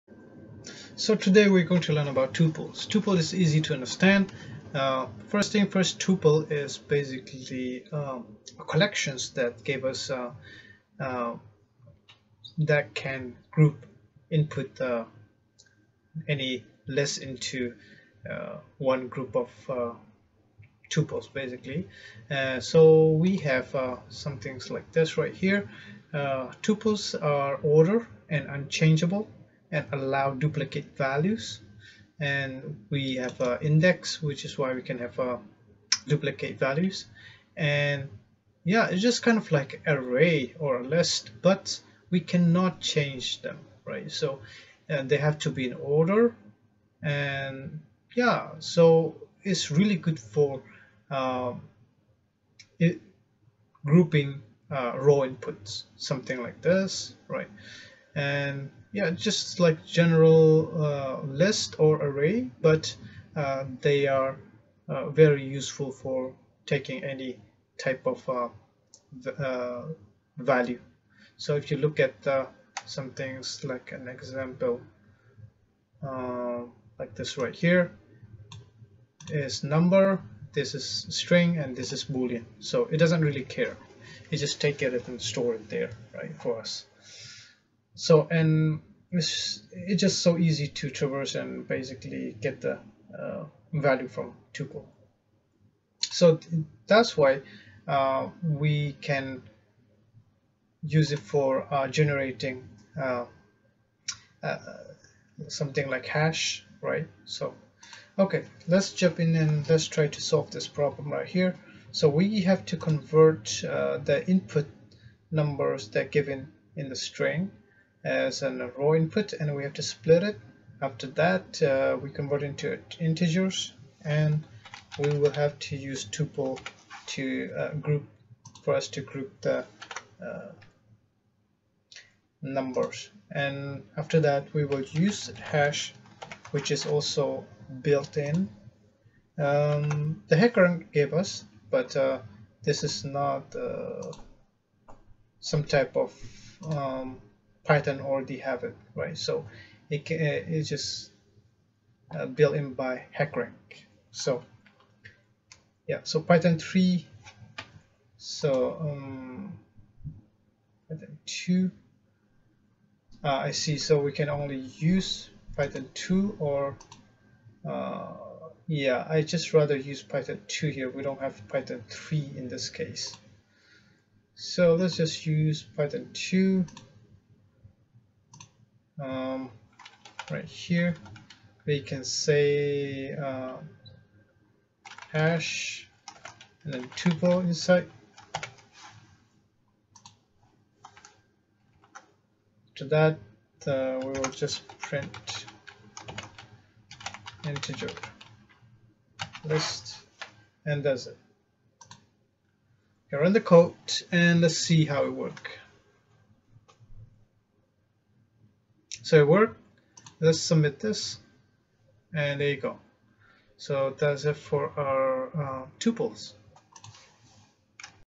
Python-Tuples
Generate the hash numbers from the given string with tuple.